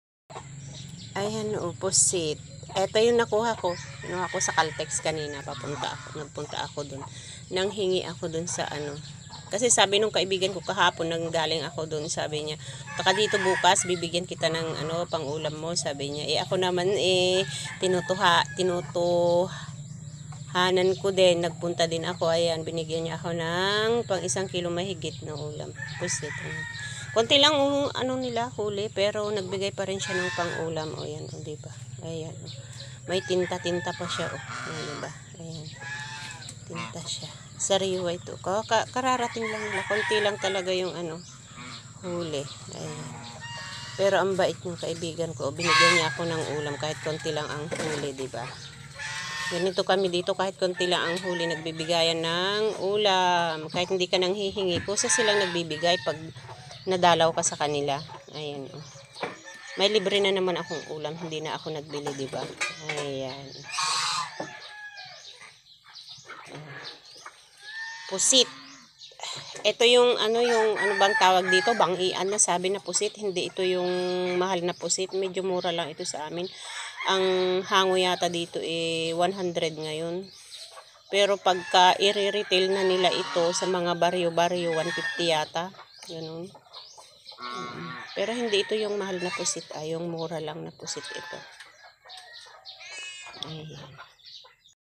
Pusit bigay ng kaibigan ,dito sa amin uso ang palitan ng ulam
Dito sa probinsya may ulam kahit wala kang pambili kasi maraming nagpapahingi ng ulam,.